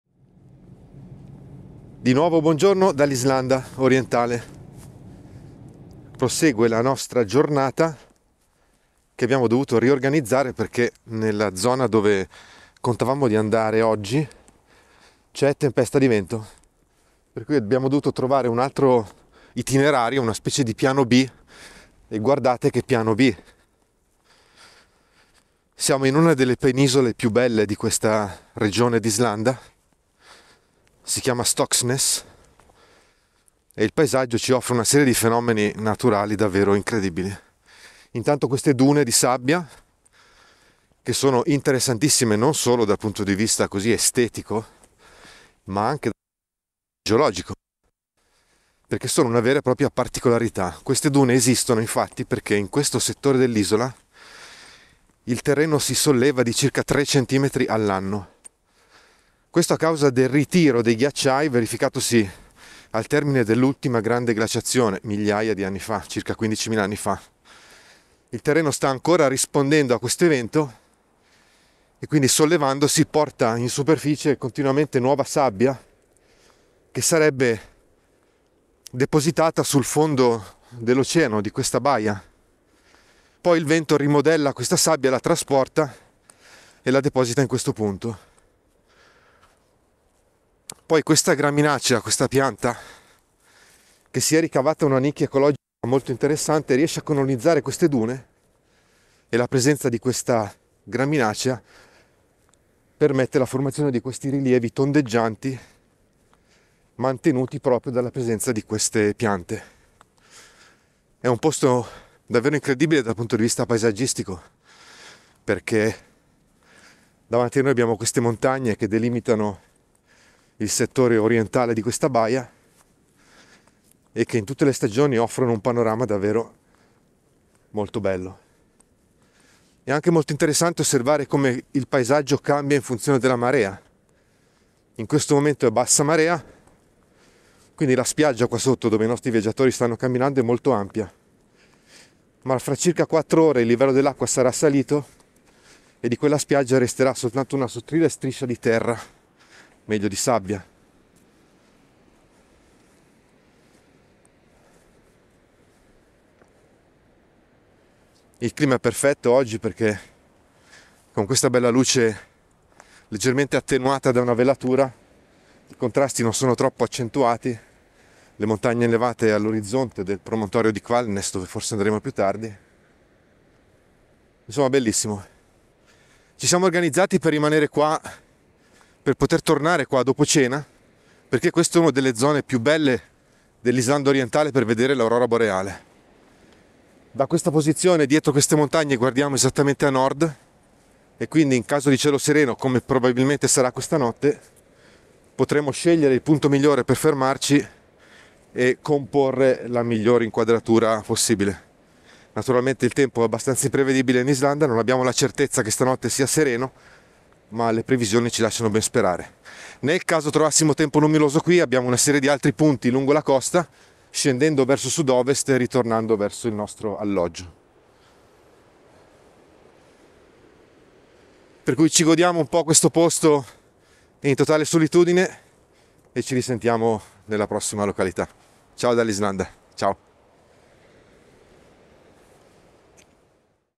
Luoghi da non perdere in un viaggio invernale in Islanda
Un viaggio in Islanda, in inverno, regala ambienti che si ricordano per una vita, non solo l'aurora boreale.
Poterli visitare nelle migliori condizioni è un privilegio riservato a pochi.
Certo, è necessario essere ben organizzati, conoscere i microclimi locali e i modelli meteorologici, costruire itinerari giornalieri che permettano di spostarsi in punti diversi senza perdere troppo tempo e immaginando quale sarà l'evoluzione meteorologica nel corso di ogni giornata.
L'esperienza è davvero completa e gratificante se si ha l'opportunità di conoscere i fenomeni che sono all'origine degli scenari che si stanno osservando.
Questo è ciò che facciamo durante i nostri viaggi i Islanda.